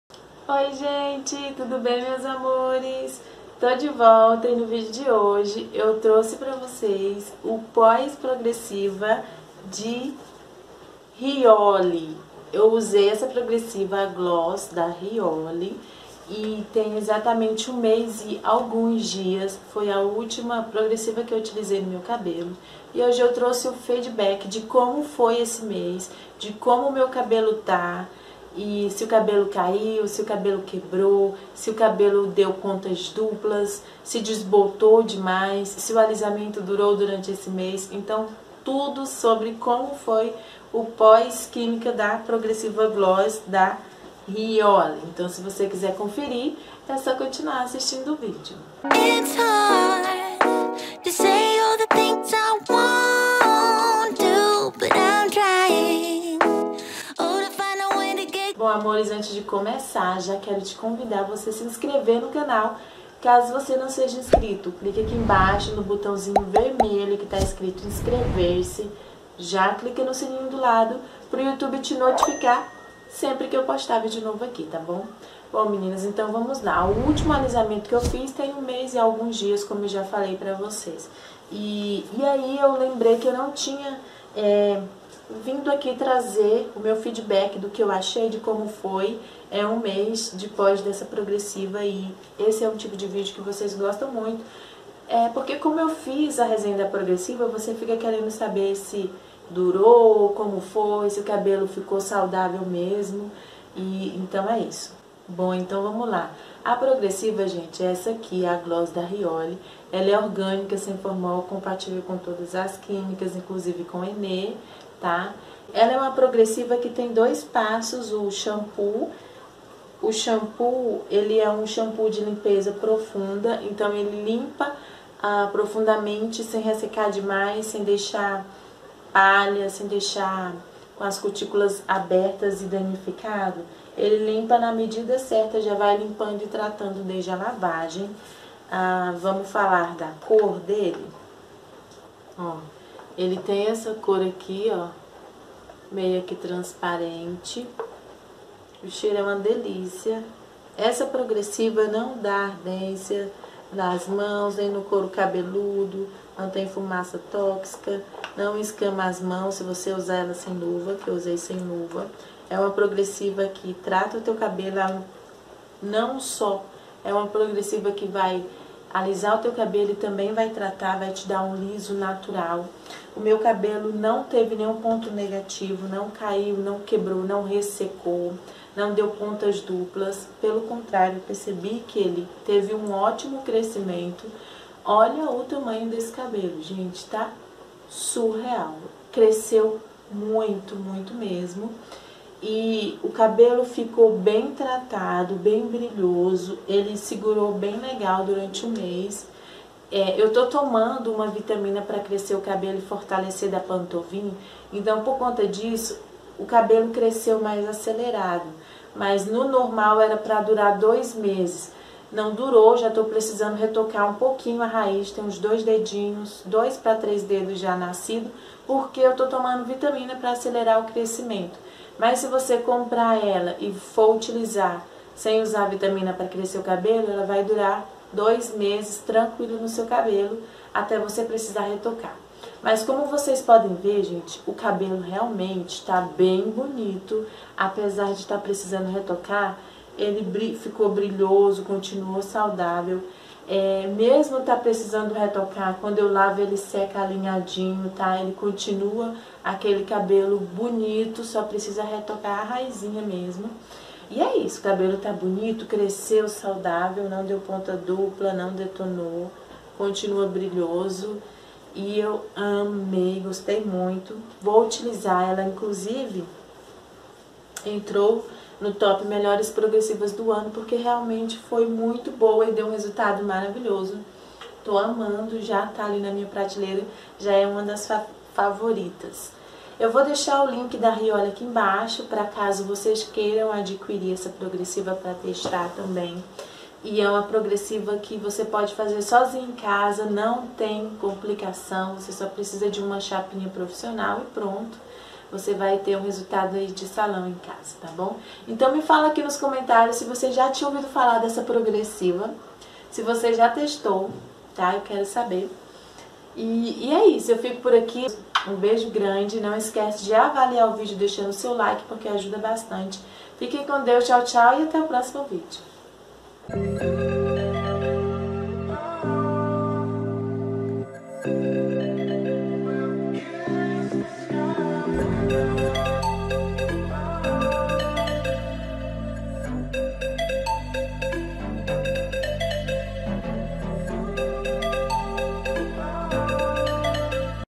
Pós Química Rioli O Cabelo Caiu Quebrou...Confiram aí no video/ Andréa Monteiro
progressiva alisamentos progressivaorganica progressiva alisamentos progressivasemformol melhorprogressiva ♥ Oi amores relato sobre como ficou meu cabelo depois de usar a progressiva da Rioli, Será que caiu, ou quebrou? Conferi os detalhes ai no vídeo. Lembrando que temos vídeos novos toda 3º e 6º feira, se inscrevam pra não perder.
&&&&  ESSE VÍDEO NÃO É UM PUBLI &&&&